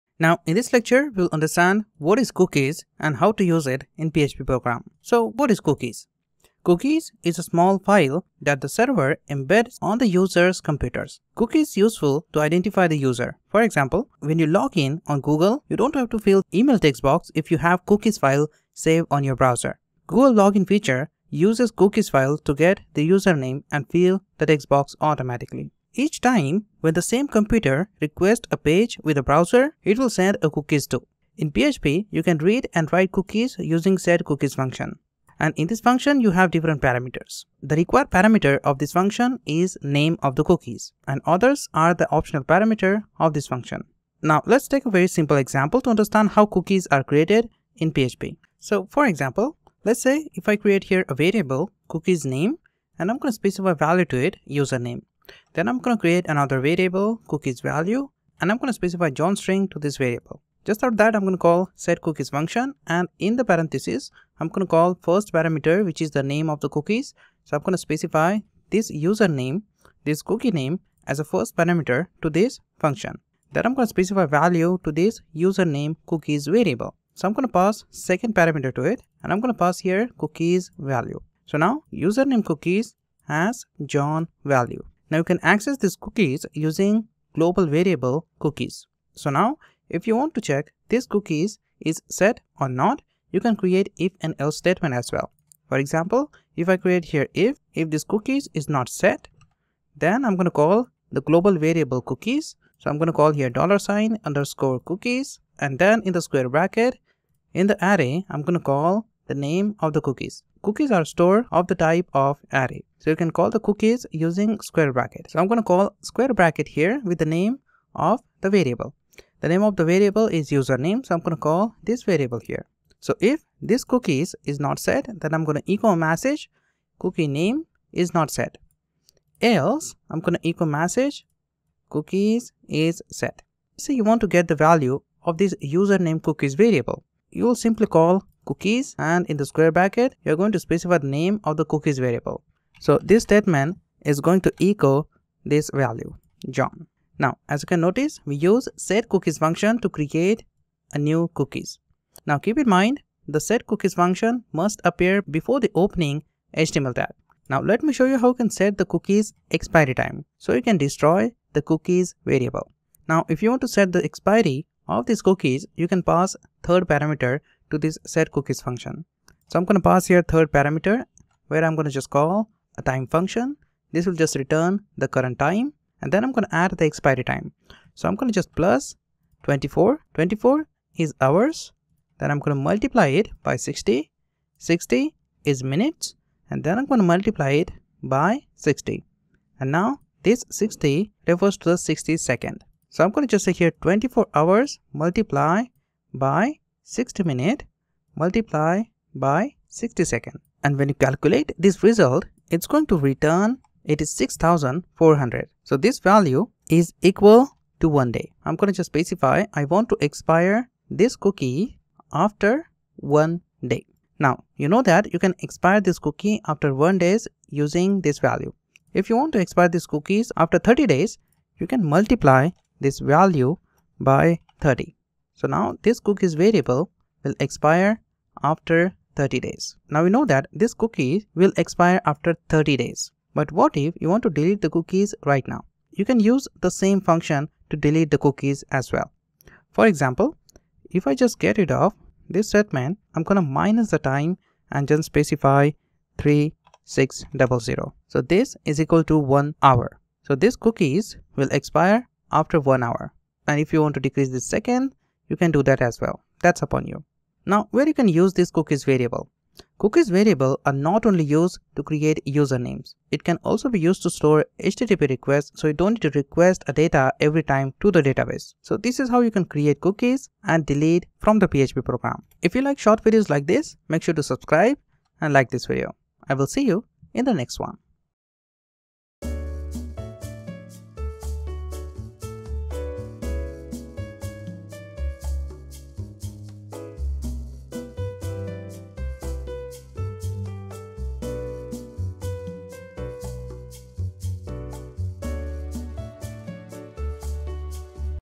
What is Cookies & How to use it in PHP - In 5 Minutes
In this tutorial, we will talk about how cookies are created. We will learn how to create cookies and make them useful.